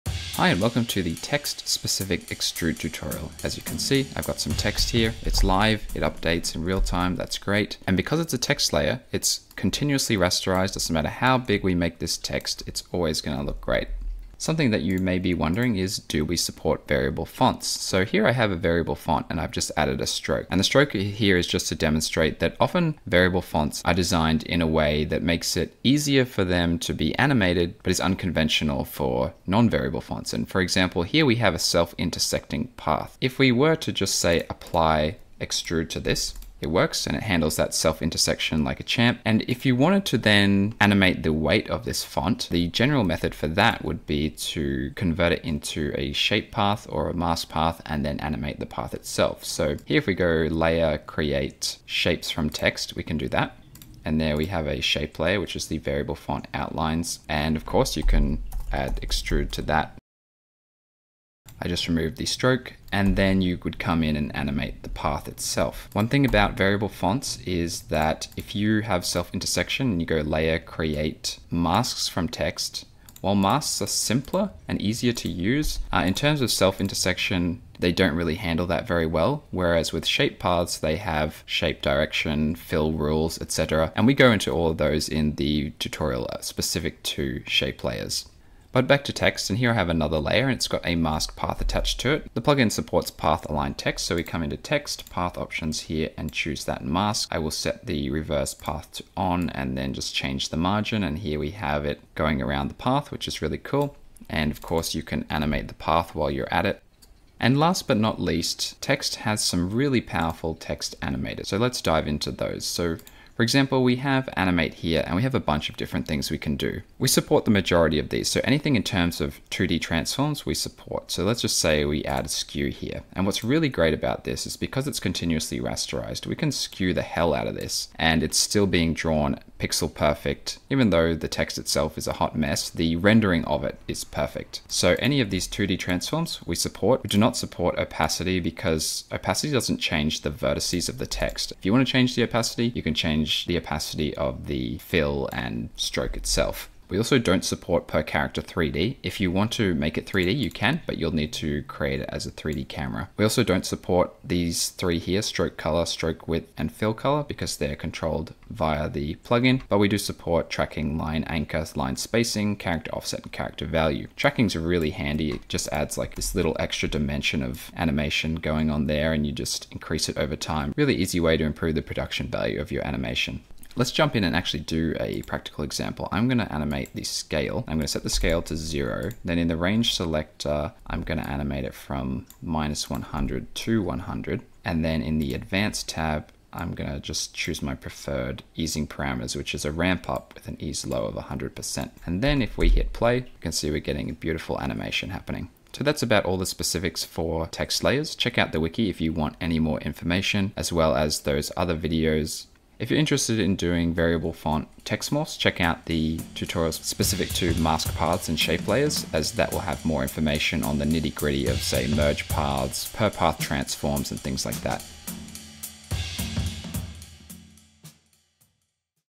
Extrude for After Effects - Working with Text Layers
This video covers all text layer related topics: variable fonts, path aligned text, text animators and text morphs. For more detailed info on every parameter as well as practical guides, best practices and FAQ check out our wiki

0:00 Introduction
0:15 Variable font compatibility
0:45 Text morphs with shape layers and mask paths
1:30 Path aligned text
1:50 Text animator compatibility
2:55 Text animators transition practical example
3:25 Further watching for text morphs with shape layers and mask paths

Bring live text layers, shapes & masks to life in a single click with Extrude. Features 2D Directional, Vanishing, or Full 3D extrusions modes.